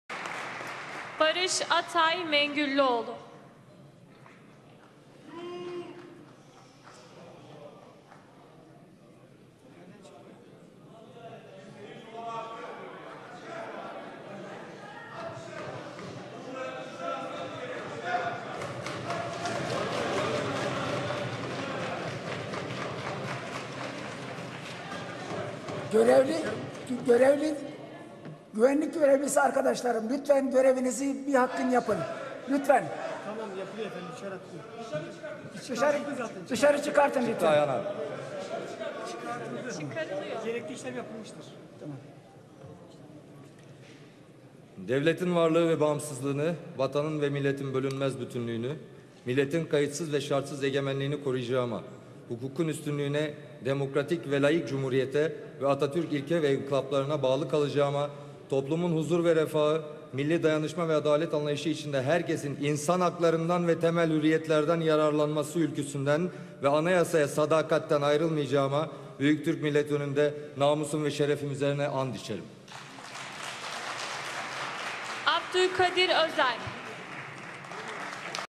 HDP'li Barış Atay Yemin Ederken TBMM'de Olay Çıktı
HDP'den Hatay milletvekili seçilen oyuncu Barış Atay Meclis'te yemin etti. Atay yemin etmek için kürsüye gelirken, salonda bulunan bir kişi önce yuhaladı, ardından da kalabalık bir grup Atay'ı protesto etti. Meclis Başkanı Yılmaz, güvenlik görevlilerine protestocuları dışarı çıkarmaları talimatı verdi. Atay, protestocular dışarı çıkarıldıktan sonra yemin etti. TBMM'de milletvekillerinin yemin töreninde kısa süreli olay çıktı. HDP'den Hatay milletvekili seçilen oyuncu Barış Atay, yemin etmesi için anons edilince TBMM Genel Kurul salonu bir anda karıştı. Yemin törenini izleyen bir kişi, önce Atay'ı yuhaladı. Ardından da kalabalık bir grup kürsüye doğru giden Atay'ı protesto etti MECLİS BAŞKANI: GÜVENLİK GÖREVLİLERİ GÖREVİNİ YAPSIN en yaşlı üye sıfatıyla Meclis'e başkanlık yapan Durmuş Yılmaz, protestoların ardınadn güvenlik görevlilerine talimat vererek "Güvenlik görevlisi arkadaşlarım, lütfen görevinizi yapın" dedi ve protestocuların dışarı çıkarılmasını istedi. Daha sonra tepki gösteren kişi TBMM genel kurul salonundan dışarı çıkarıldı. Atay, daha sonra yemin ederek görevine başladı. YEMİN EDERKEN AK PARTİ SIRALARINA BAKTI Barış Atay'ın, yemin metnindeki "İnsan hakları" kısmını özellikle vurgularken, bu sırada AK Parti sıralarına baktığı görüldü